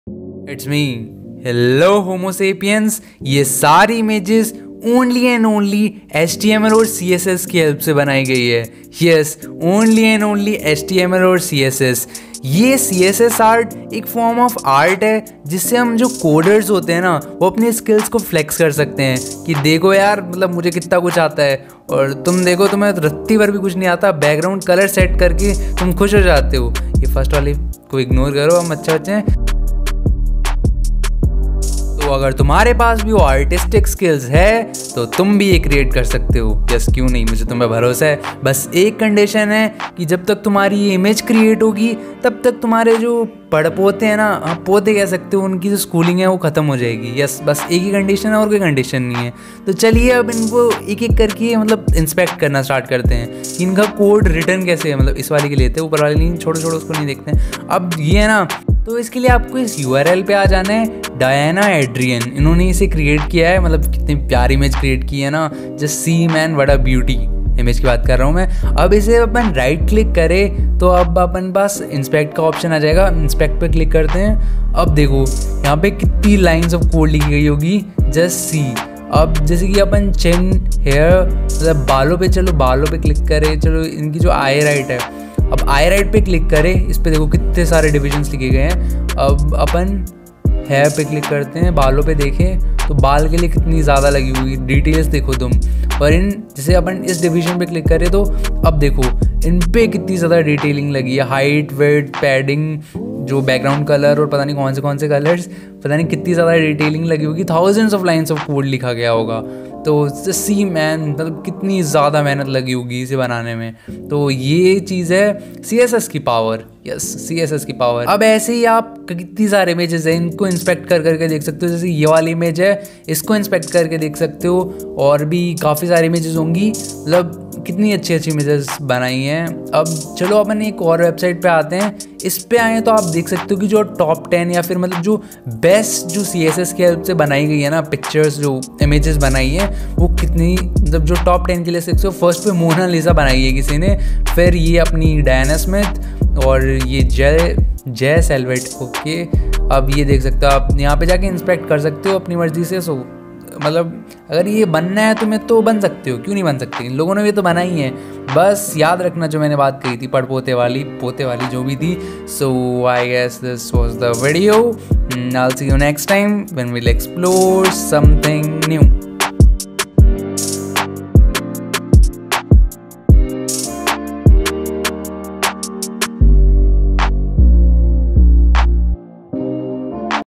Beautiful Art made using HTML and CSS
HTML and CSS can be used to create beautiful art. CSS Art is a fun way for coders to flex their tech skills and their creativity at the same time. CSS Art involves writing code that will display an object in a browser and then using shapes, gradients, and lines to create a digital image that is built purely with HTML and CSS. CSS Art can seem overwhelming at first, but once you see more examples and their code, it’s easier to see how an image can be broken down into small sections(still crazy efforts to make it) . There are many examples of impressive CSS Art projects that are made purely with CSS, including portraits, landscapes, playing cards, and even realistic portraits. CSS Art is worth checking out if you’re looking to become more familiar with CSS or get more creative with your coding.